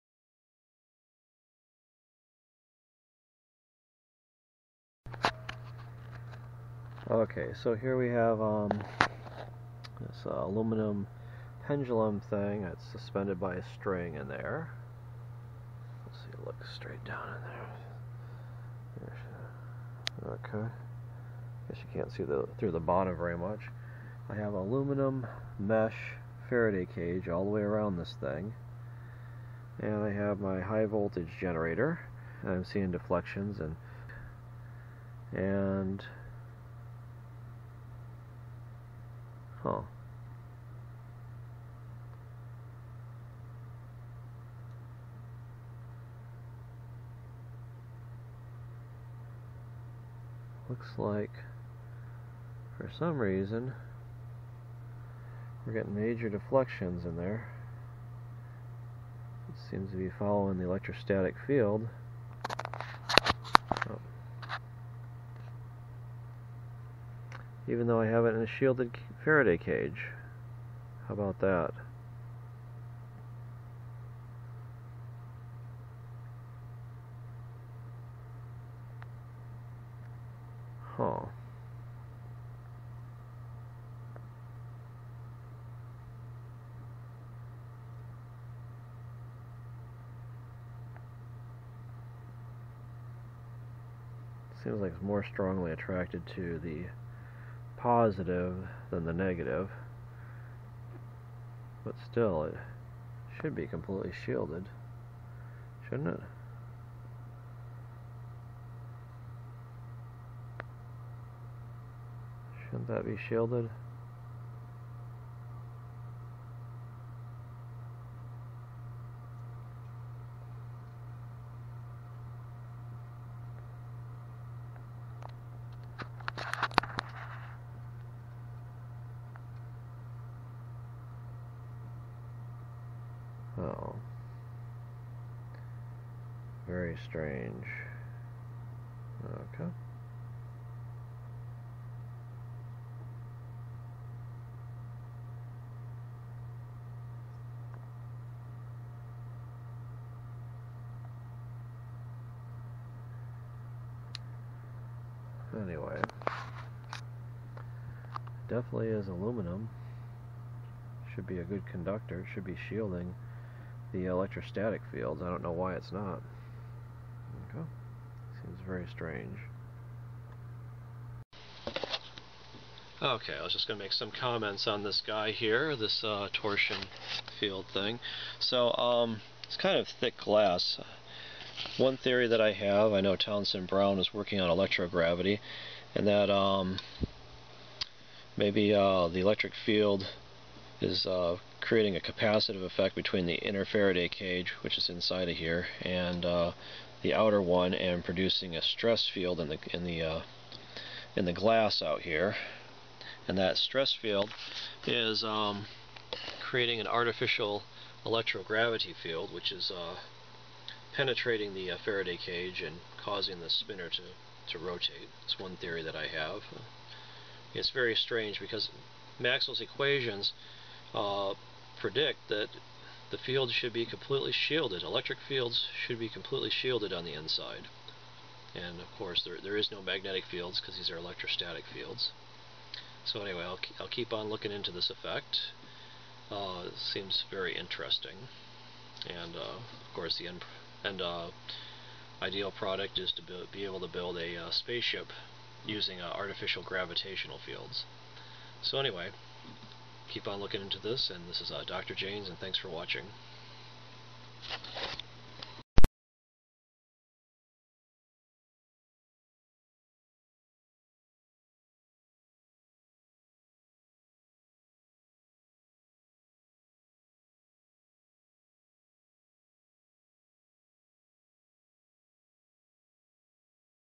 Electro-Gravity Torsion Pendulum
Here is an electrogravity experiment conducted on a torsion pendulum, electrostatically shielded inside a Faraday cage. The gravitational like force seems to penetrate the metal cage and exert a force upon the aluminum torsion pendulum within.. The field is a high voltage DC electrostatic field generated by a little high voltage multiplier. The results are reminiscent of the research done by Townsend Brown, on flying disc propulsion systems.